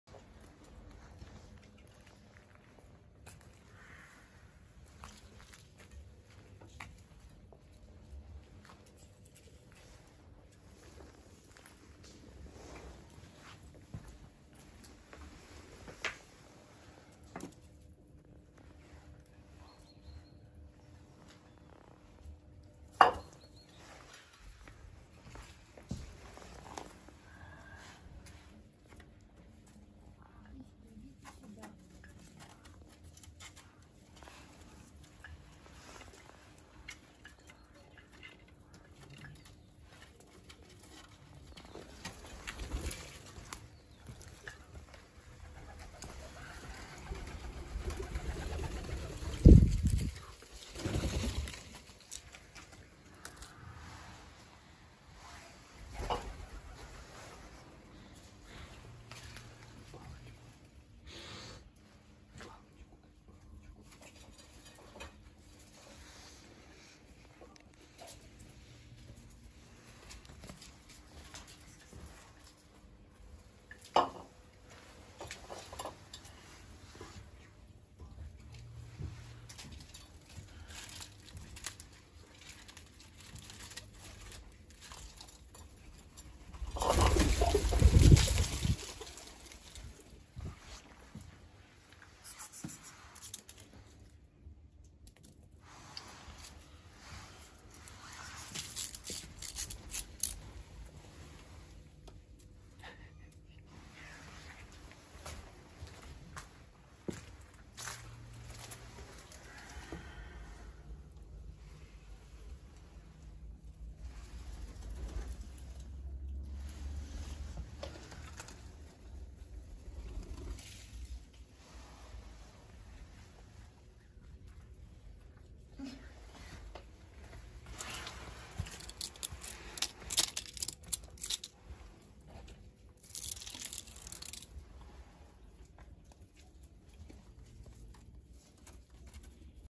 Two scared kittens and a pigeon. Cute homeless kittens. Ginger kitten
Who has the desire and ability to help kittens and cats.

sweetest kittens.
the cutest kittens videos.
crying kittens.

Two scared kittens and a pigeon. Cute homeless cats